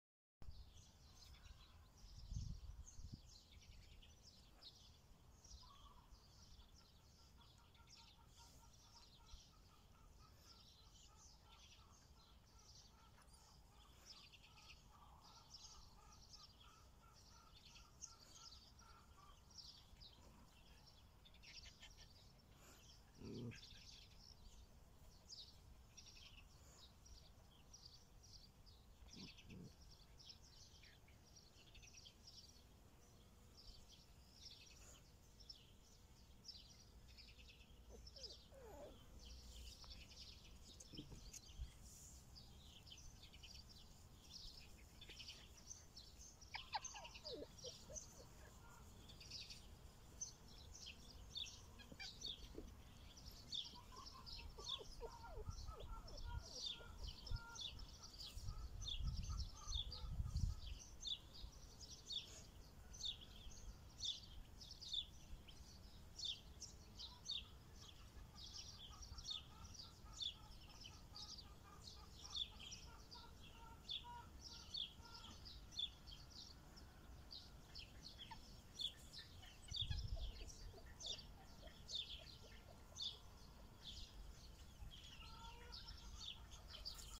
🥰🐕 Lots of cute and beautiful dogs.
❗When there was peace in Ukraine, I had a dog kennel.  I love dogs so much.  Now my dogs live in different countries with my friends.  I really want peace in Ukraine.  I really want dogs to live in my house again.  But now it's very dangerous ❗